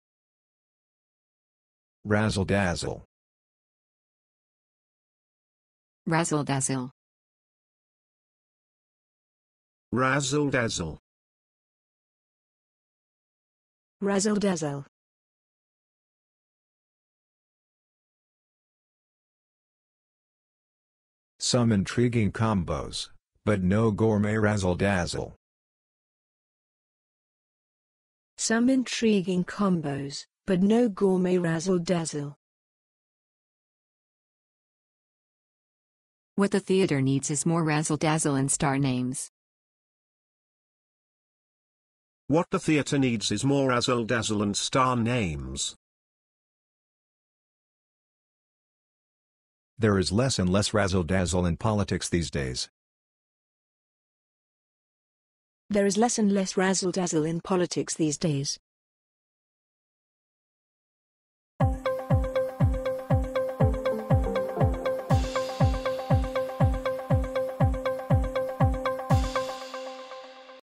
RAZZLE-DAZZLE pronunciation | Improve your language with bab.la
Improve your spoken English by listening to RAZZLE-DAZZLE pronounced by different speakers – and in example sentences too. Learn and love languages with bab.la.